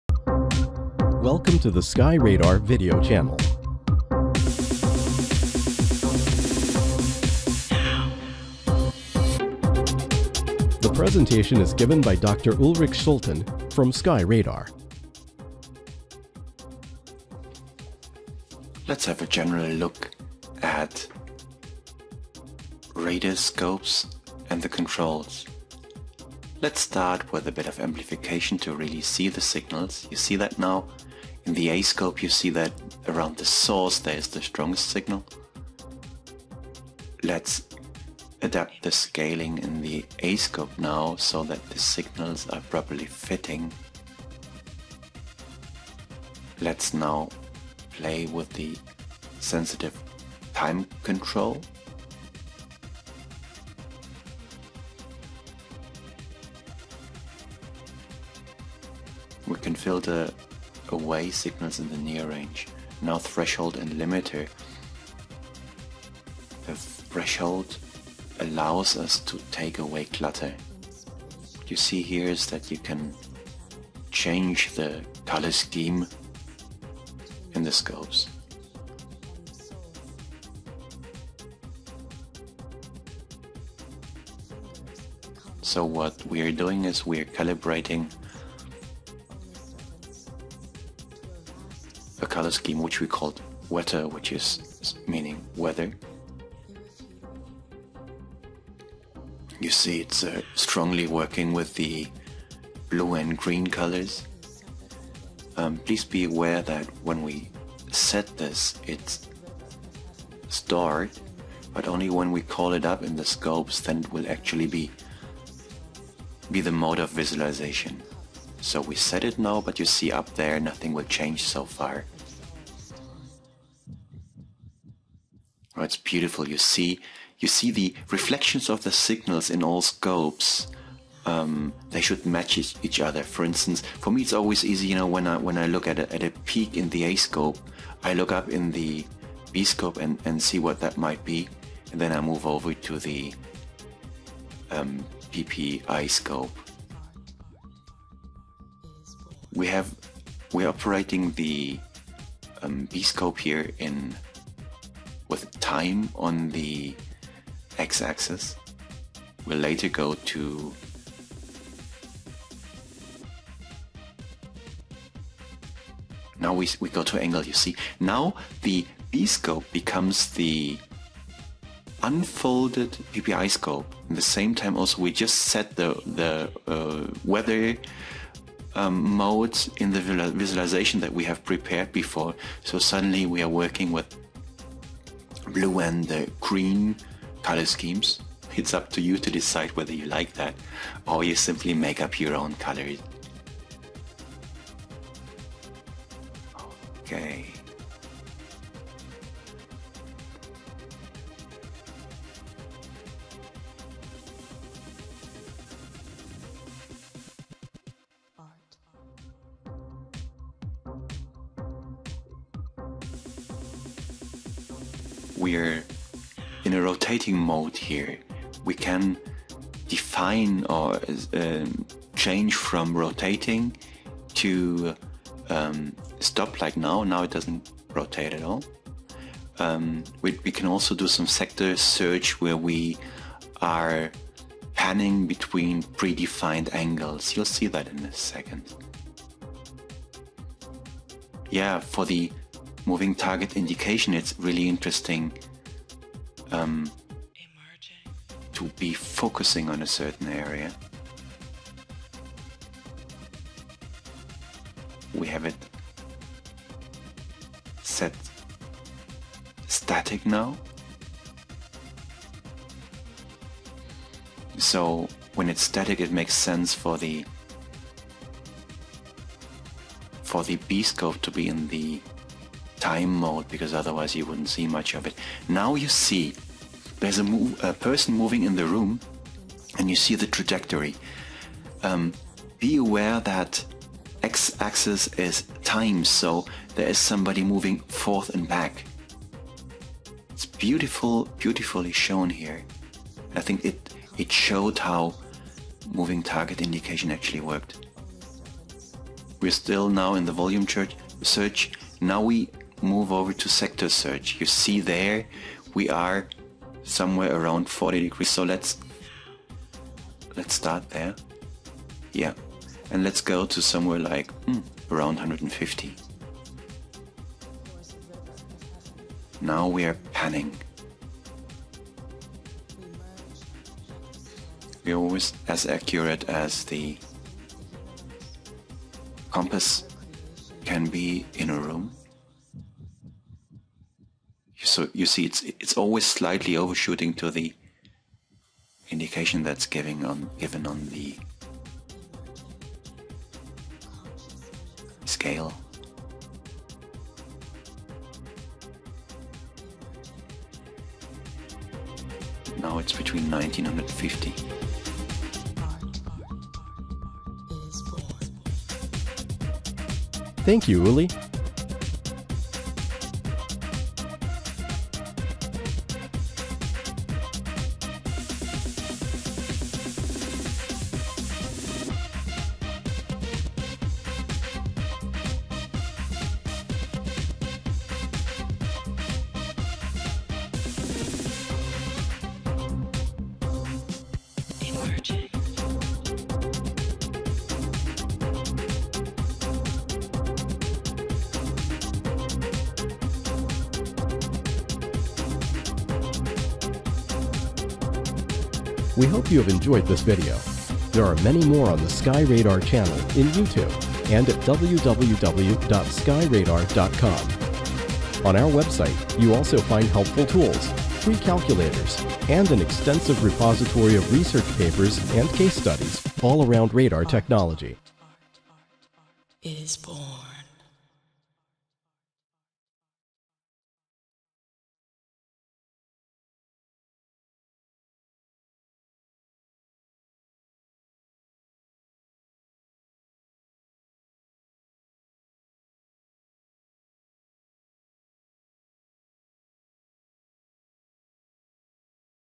SkyRadar Webinar: General Introduction into scopes of Primary Surveillance Radars
This video gives a general introduction into the scopes of PSRs. It talks about A-scopes, B-scopes, PPI, Sensitive Time Control STC, Moving Target Indication MTI, Tracking, Rotation and Panning and more. All is shown at the example of SkyRadar's close range training radar PSR Base Unit.